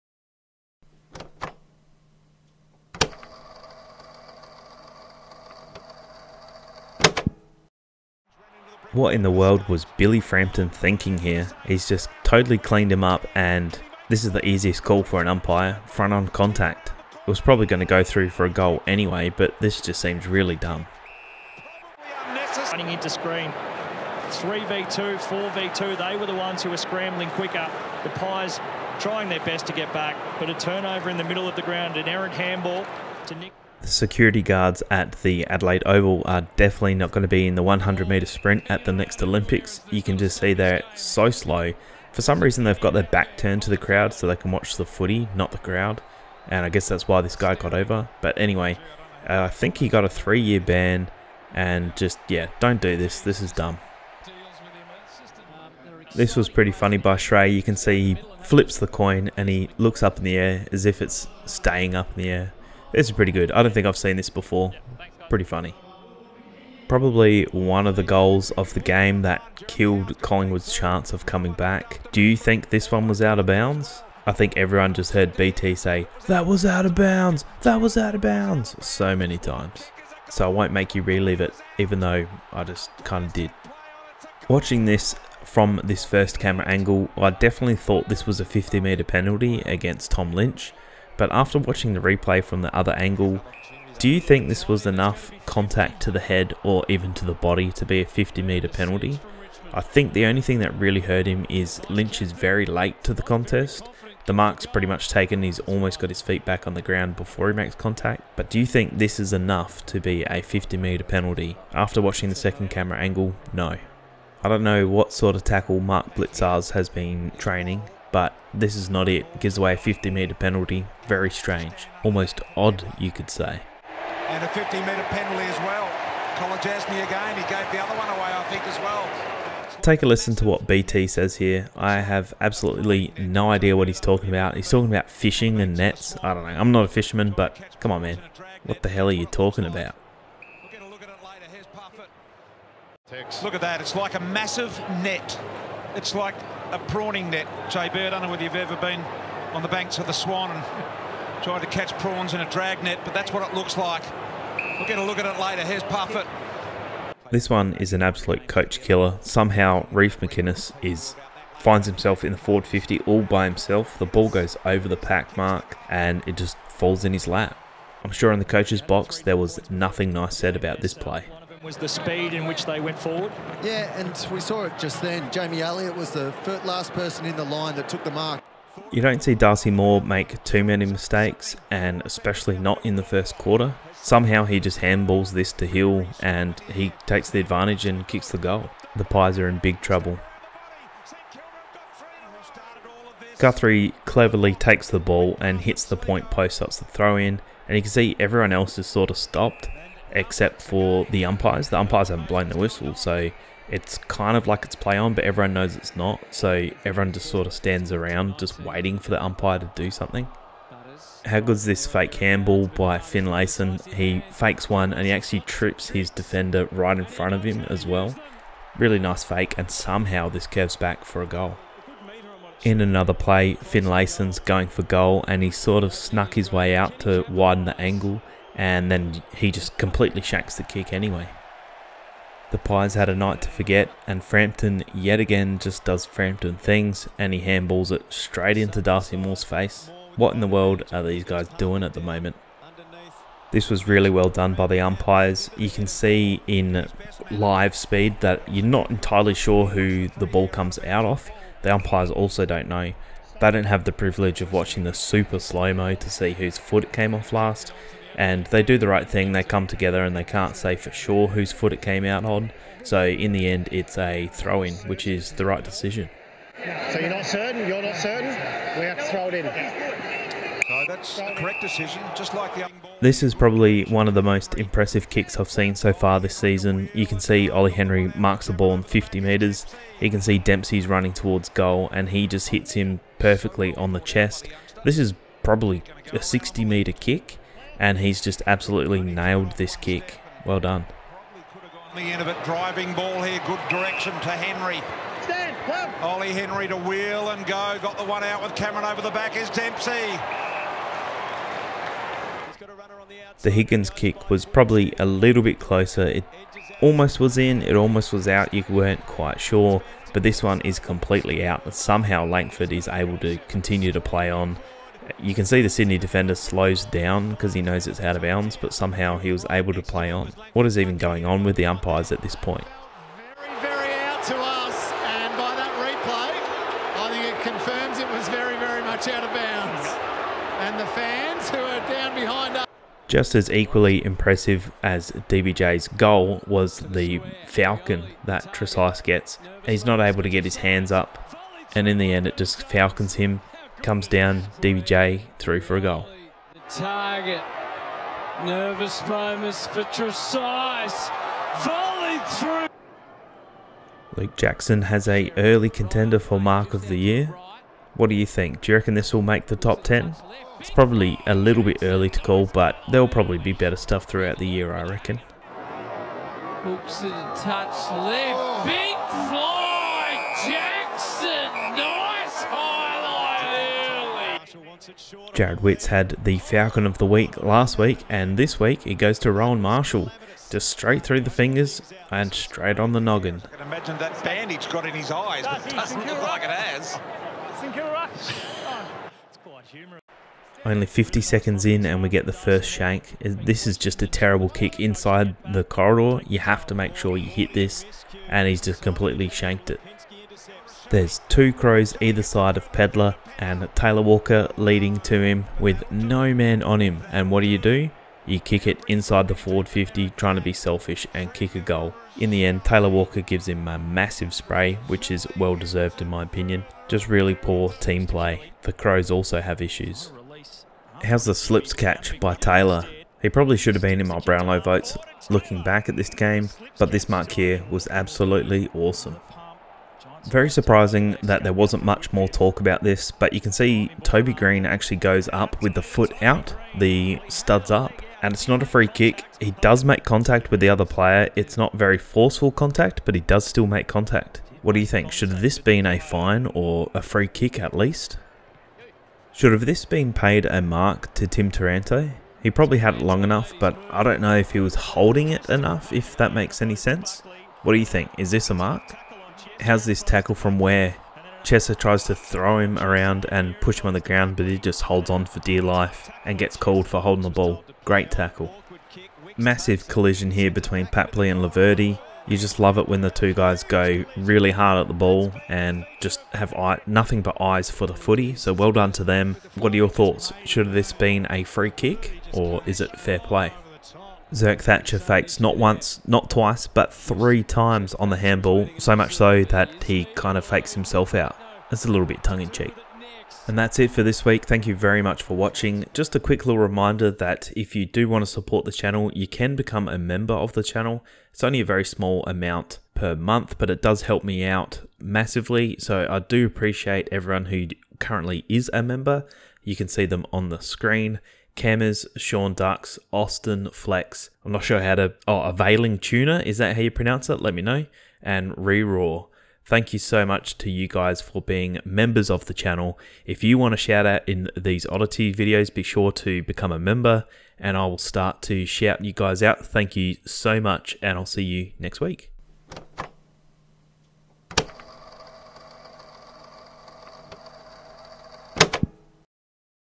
Oddities In The AFL Round 2 2024
MASSIVE THANKS TO:
austin
JustRandom [GD]
Benn Polomka
availingtuna
Ryan
Reroar
jon_redding2007 Archives
James Hicks

Let's take a look at some of the odd things that we saw in the AFL Round 2 games. Still plenty of odd things that happened in our game this week. What is your favourite oddity??

Timeline -
00:00 - Big Thanks
0:15 Subscribe